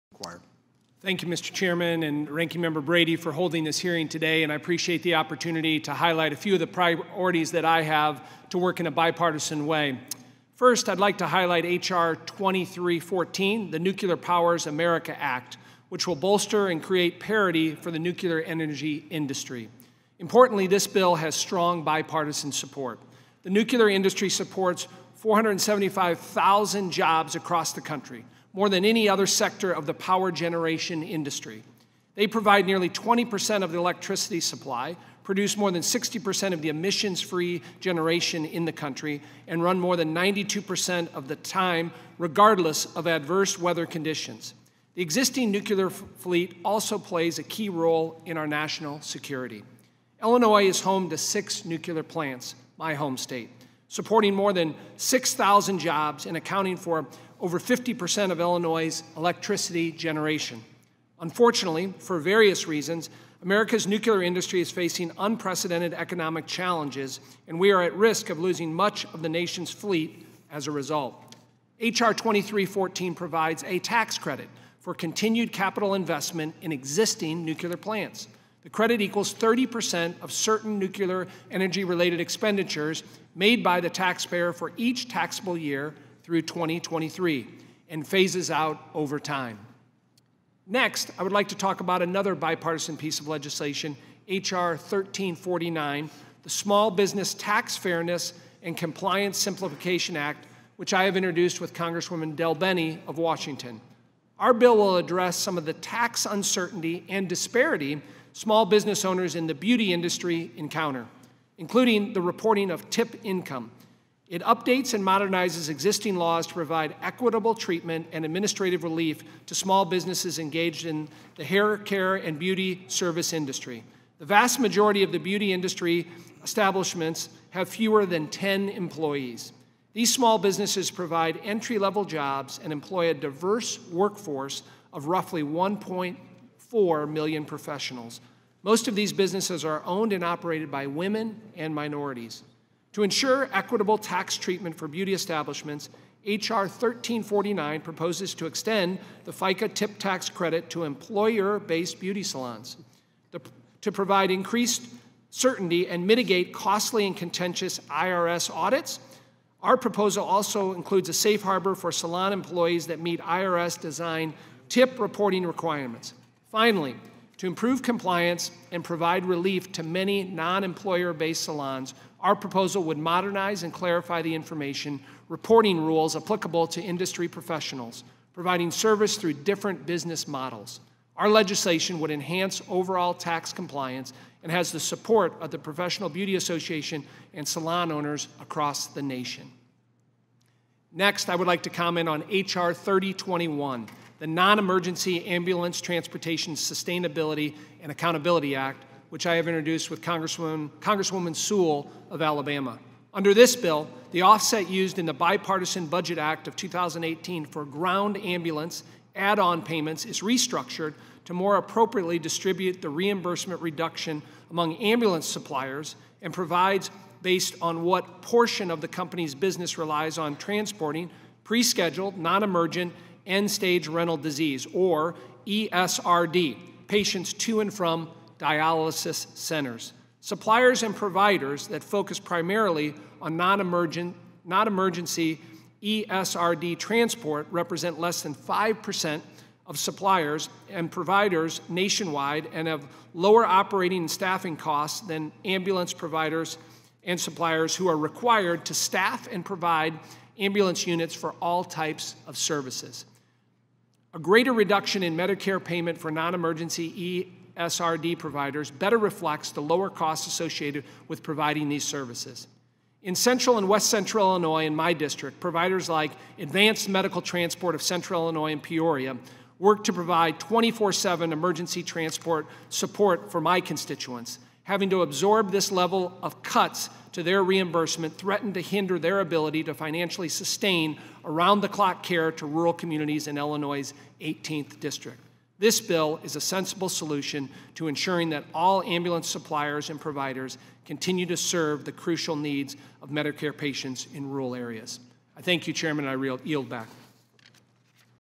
Rep. LaHood Speaks at Ways and Means Member Day Hearing
Congressman Darin LaHood spoke at the Ways and Means Committee's Member Day Hearing on legislation he has introduced that will support communities in central and west-central Illinois.